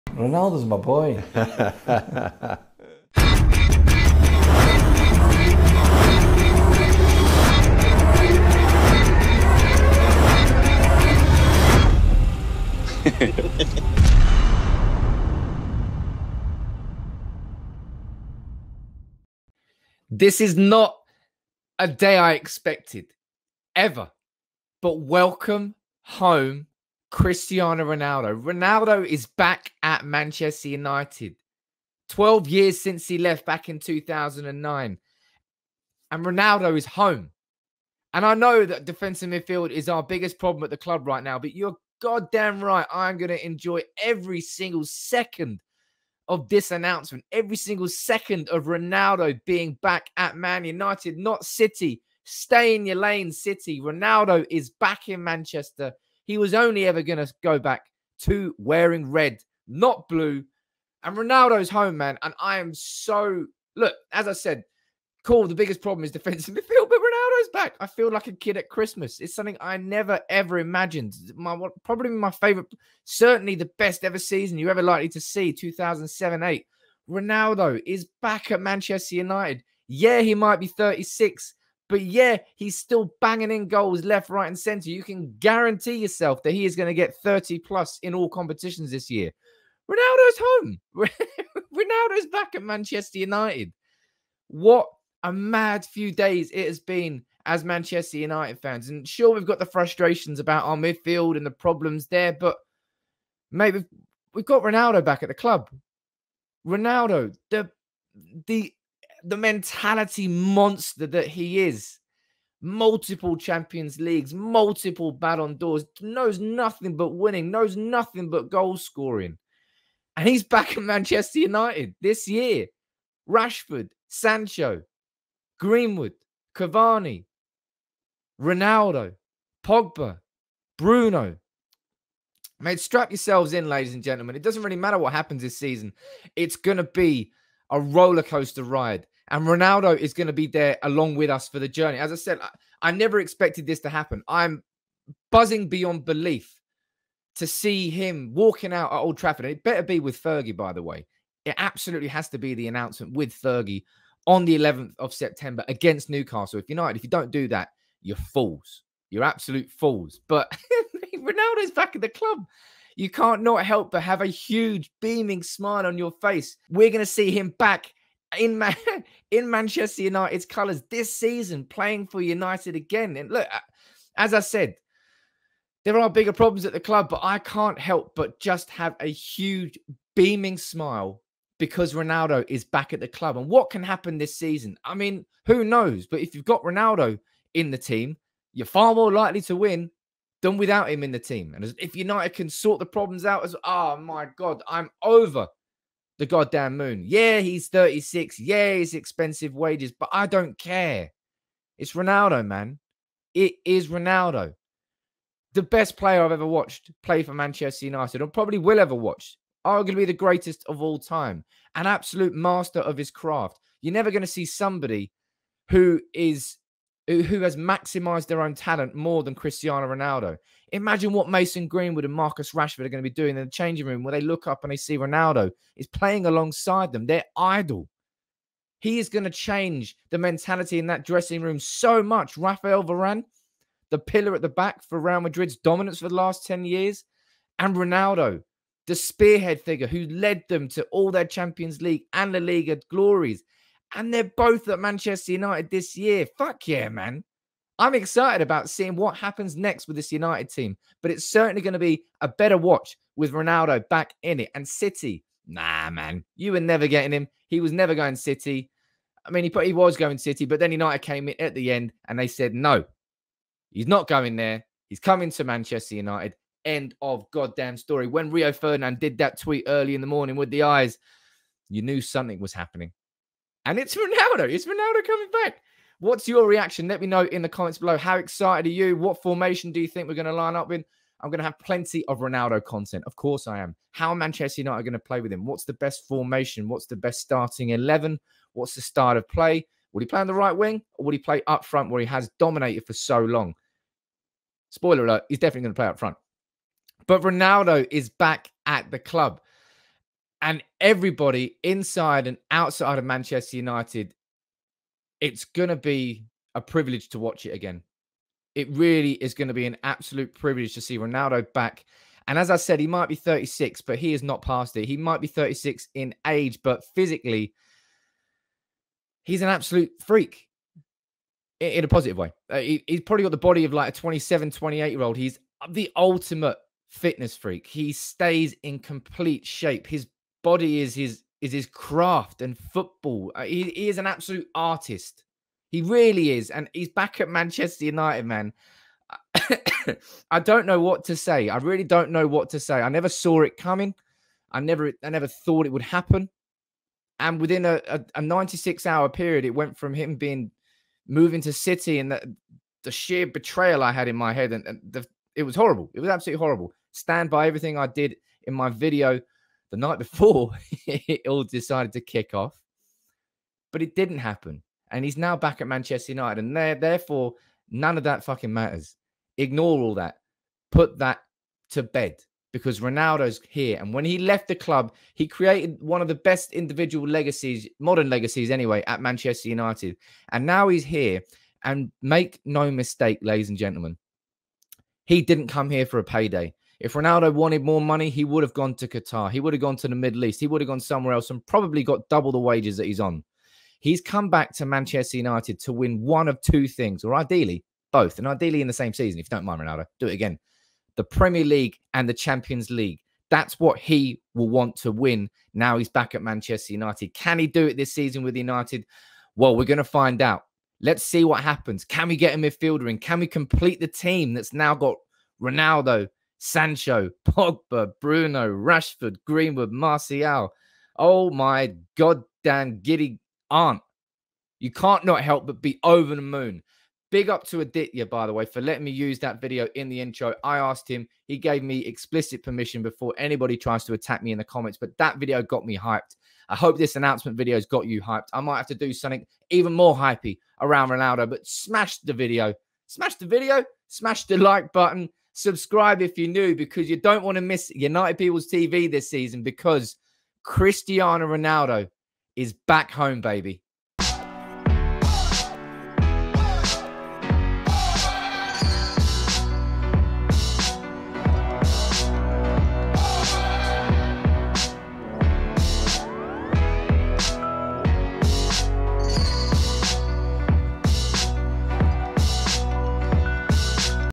WELCOME Home Cristiano RONALDO | Man United Announce Return Of Ronaldo!
Cristiano Ronaldo has officially re-joined Manchester United! It's been 12 years since Ronaldo called Old Trafford home but now he's back - welcome back Ronaldo!

In 895 career appearances, Ronaldo has contributed 903 goals and assists and he is now back at United with only one thing in mind - winning the Premier League and Champions League.

Still can't really believe that Ronaldo has come back to United and I can't wait to see him make his debut against Newcastle on 11 September. Viva Ronaldo!